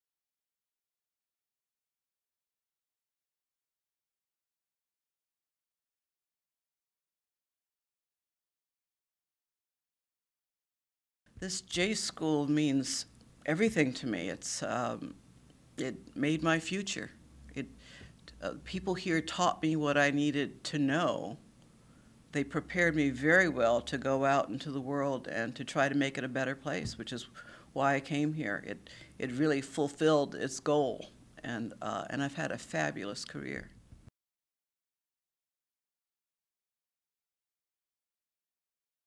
Dorothy J. Gaiter: What the J-School Means to Me
Dorothy J. Gaiter, BJ '73, retired recently from The Wall Street Journal, where she wrote the popular Tastings wine column with her husband. While a student at Missouri, Gaiter served as one of the founding editors of Blackout, a newspaper published by the University of Missouri's African-American students, and wrote for The Savitar, the MU yearbook. Following graduation, Gaiter worked as a reporter at the Miami Herald and an editor at the Miami News before joining The New York Times as a reporter for the Week in Review section, the metro desk and the style section. In 1984, Gaiter returned to the Miami Herald, where she became the paper's first African-American female editorial writer and regular op-ed columnist. In 1990, Gaiter became a reporter for The Wall Street Journal in New York City, and by 1996 she had become the Journal's national news editor in charge of race and urban affairs coverage. Her writing on race was nominated twice for the Pulitzer Prize, and she won awards from the Newswomen's Club of New York and the National Association of Black Journalists. In this segment, Gaiter talks about what attending the J-School meant to her.